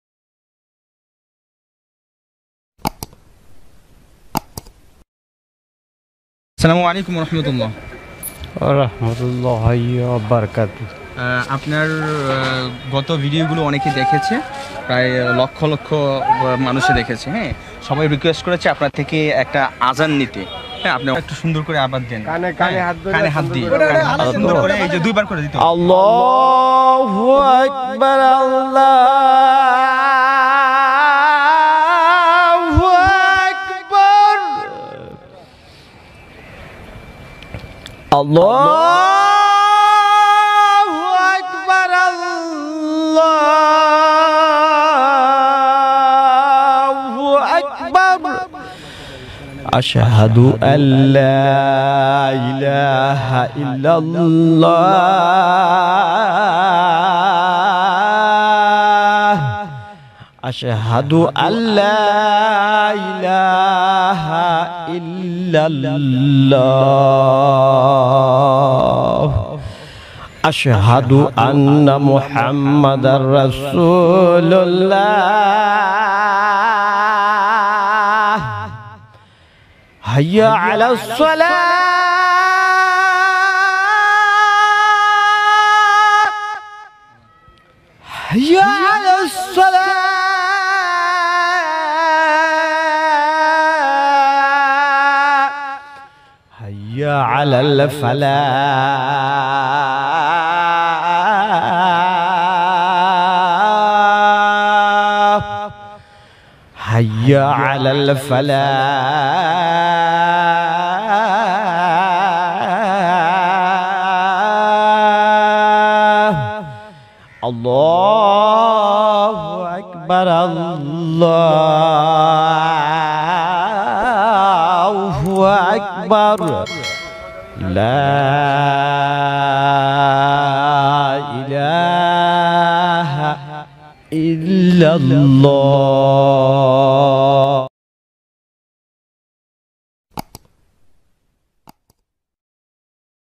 পাগলে_ আজান _দিলো _যে কন্ঠে।মানুষ _শুনে _তাকিয়ে _রয়েছে।
azan is very nice. he is a brand out.our channel is Islamic contain of hamd nath waz gogol programme runing.